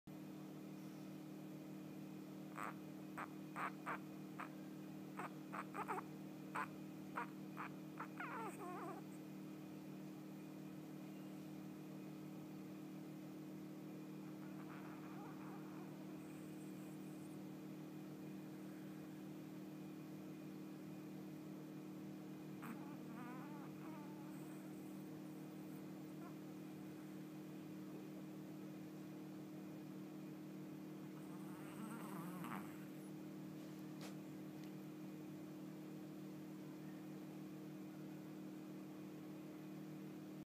sleeping chihauhau チワワ、白目で熟睡
チワワ、白目で寝息たてながら熟睡中